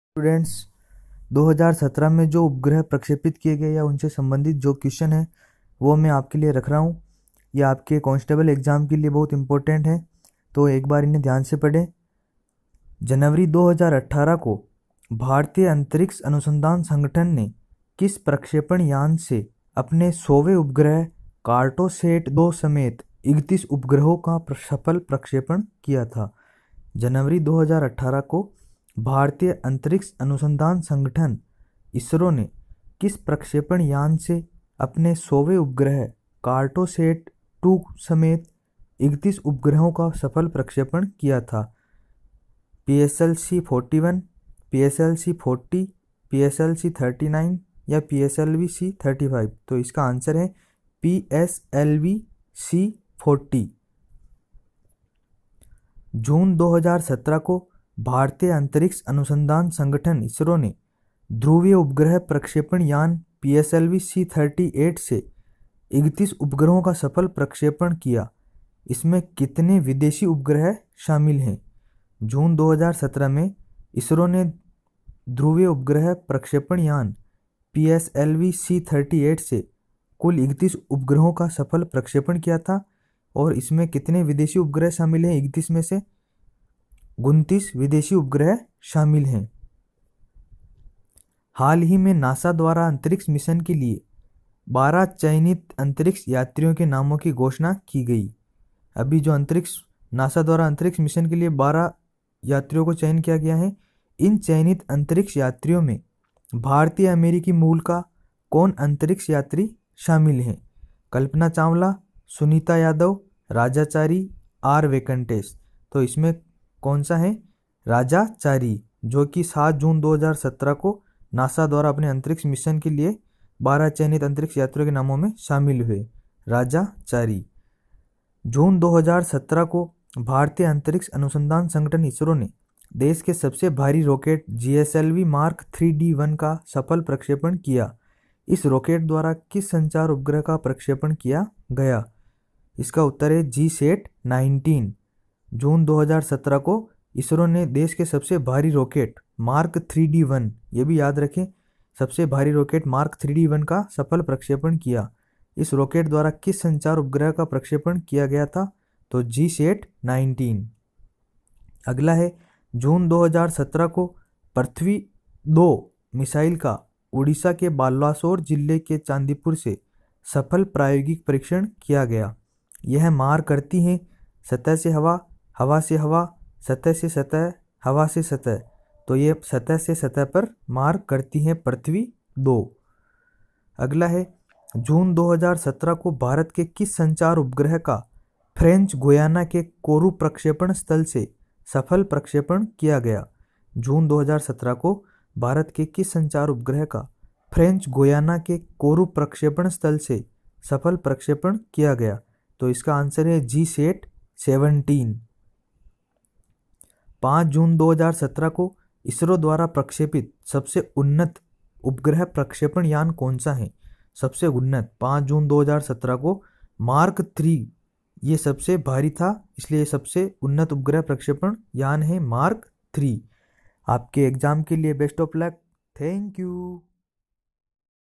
उपग्रह प्रक्षेपण के प्रश्न 2017। CONSTABLE महाभर्ती के लिए। 2 मिनिट जरूर देखें। करंट अफेयर।
Best of luck all the students